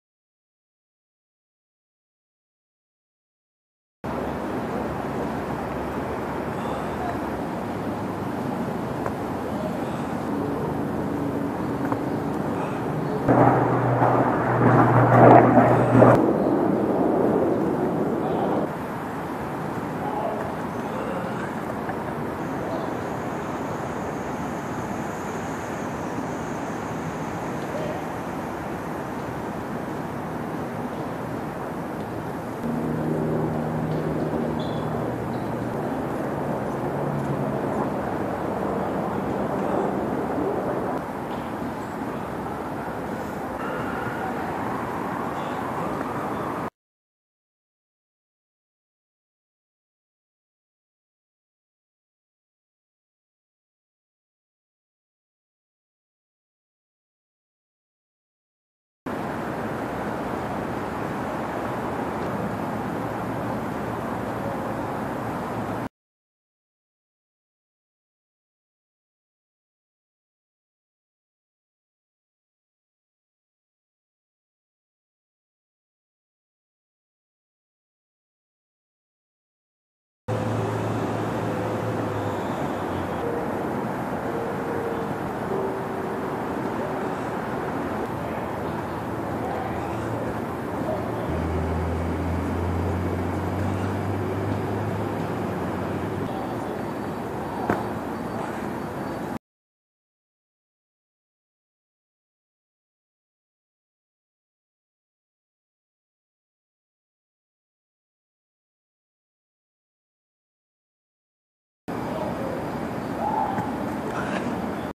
Sunset 540s
Rebuilding: converting from crescent 540 to round kick, and it's a slow change. Here's part of the progress.